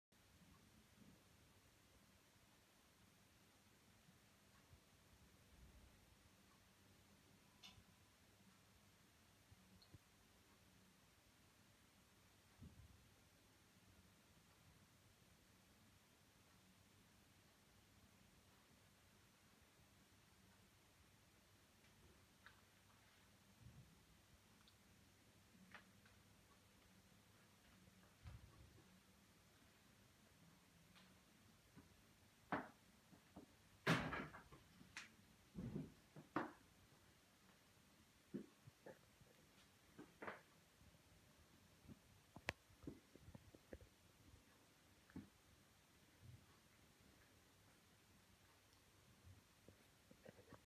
Lingholm Derwentwater Fells Modello Triptych Studio Video 2017 Rob Miller Fine Arts
A short iphone video featuring the Lingholm Derwentwater Fells Modellos in Rob Millers  studio being developed into smaller versions of the completed triptychs